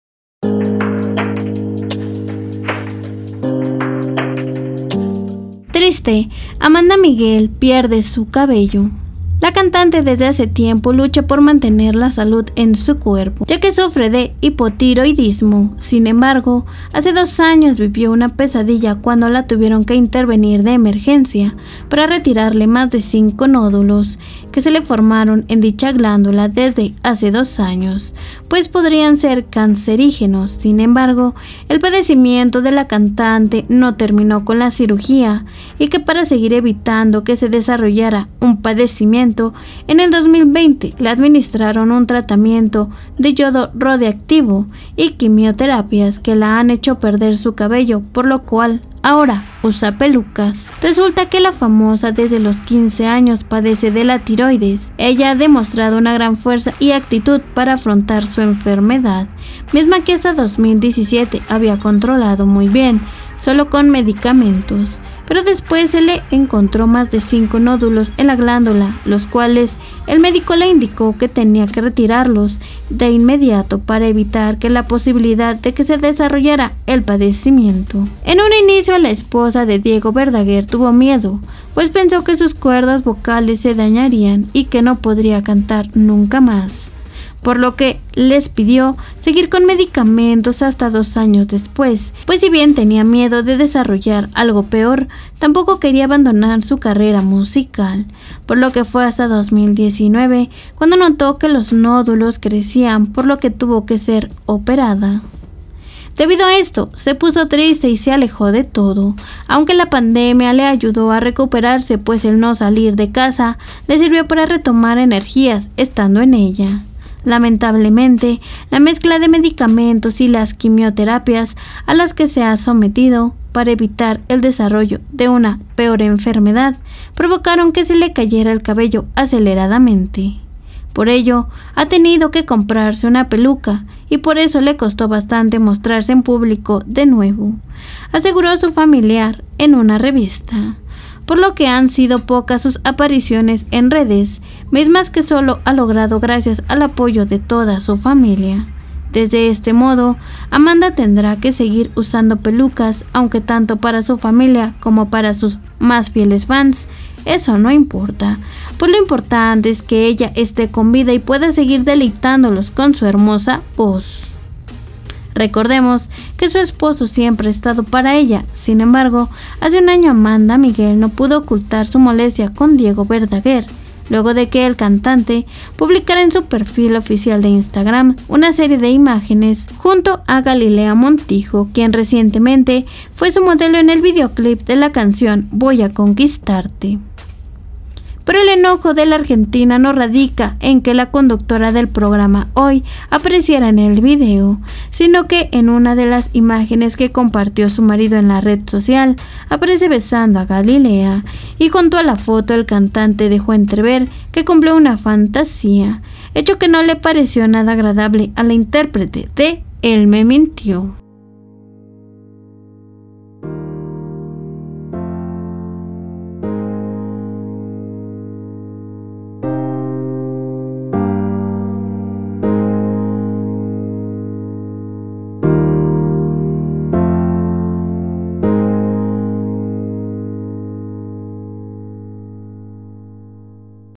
Triste, Amanda Miguel Pierde su Cabello.
Desde hace mucho tiempo estaria pasando un momento muy complicado